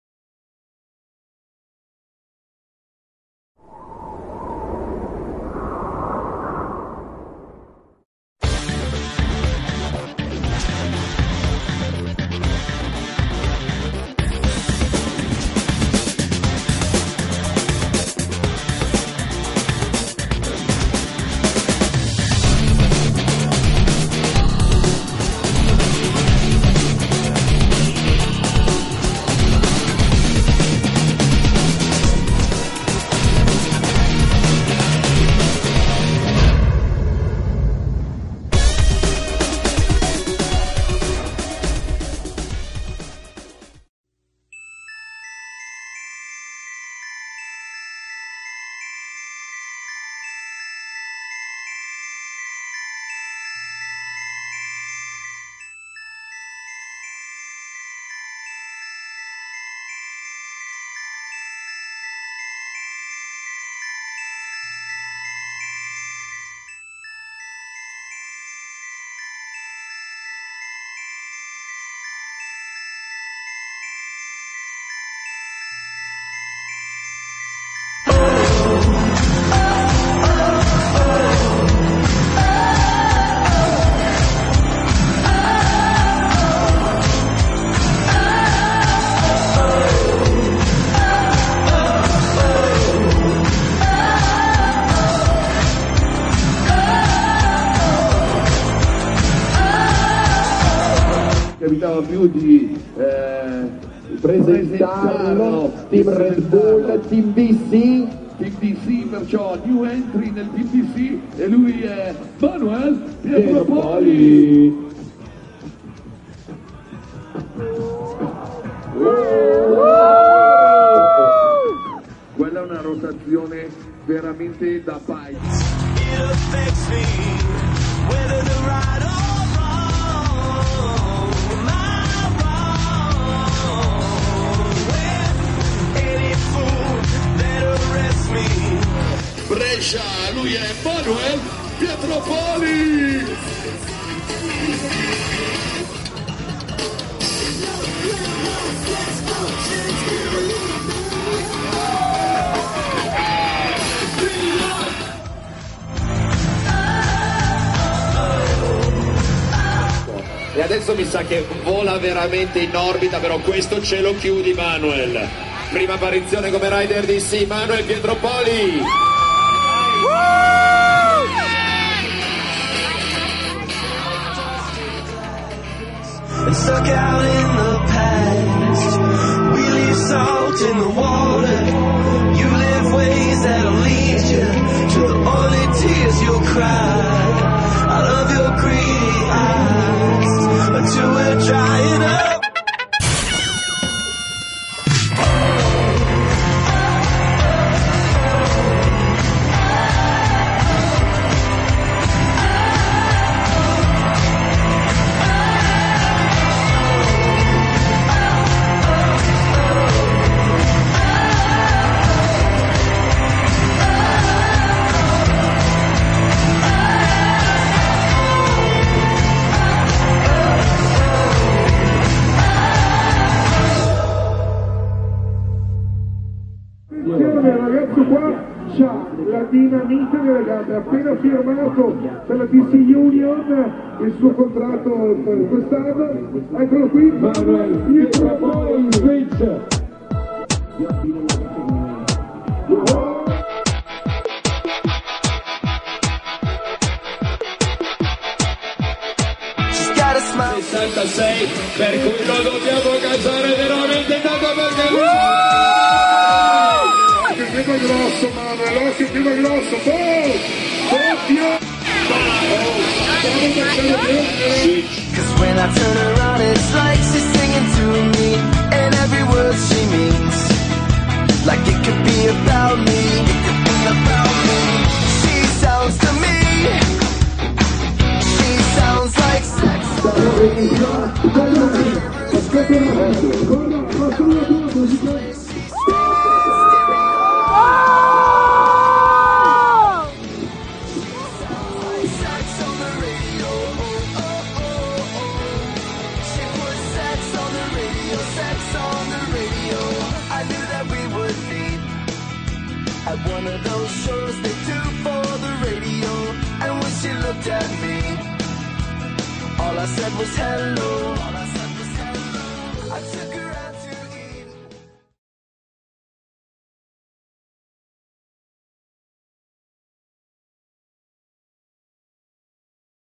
MANUEL PIETROPOLI @ SKIPASS 2011 MODENA
Manuel Pietropoli partecipa allo skipass 2011 a Modena, presentazione dei nuovi sponsor DC / UNION e gara